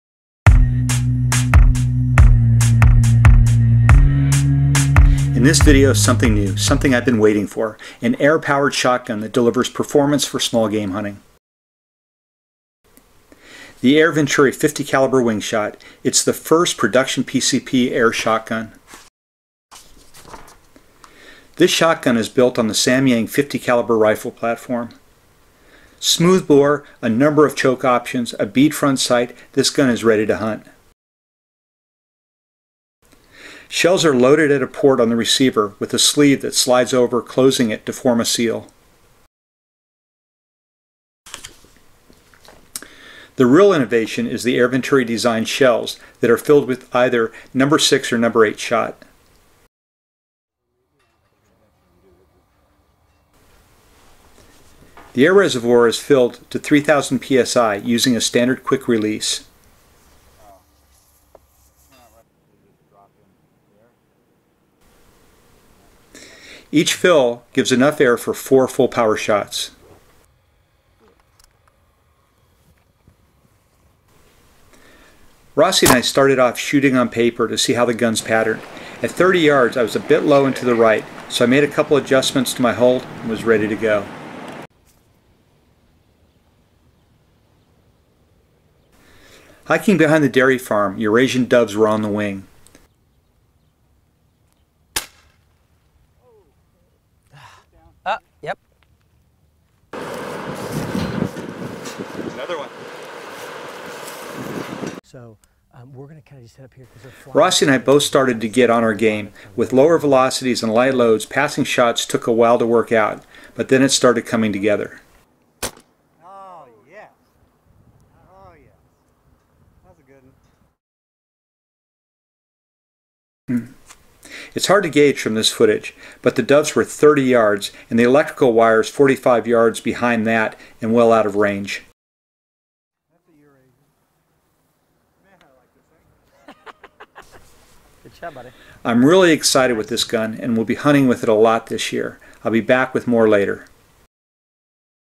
Air Shotgun Hunting Doves!
For the last couple months I've had the opportunity to shoot and to hunt with a new air powered shotgun from Air Venturi. Using a unique shot shell designed by the airgun gurus from Ohio, this .50 caliber smooth bore (under the Wing Shot moniker), gets the job done! A solid 30 yards hunting gun, this video tells you a bit about it and give a brief glimpse of the gun in action. The full hunt will air on American Airgunner on the Pursuit Network on September 9th! Be sure to tune in to see a lot more hunting action!

This air-powered shotgun take a little getting used to, because the shot strings are shorter and the velocity lower than a traditional scatter gun, passing shots took me a little while to dial in. But once that happened we were all over these fast flying birds!

Why use an air-powered shot gun? Besides just being very cool, this unique gun is much quieter than any powder burner. by a significant margin. It allows hunters to shoot in areas where either the noise or the carrying range preclude a traditional shotgun.

The shotguns we were using on this hunt were two of the first test guns before full production kicked in. The production stock has a much more traditional stock design and sighting system. I am using the first production gun to hunt squirrel, rabbit, and more birds at the present time. But in the meanwhile, take a look as we go after doves on the wing!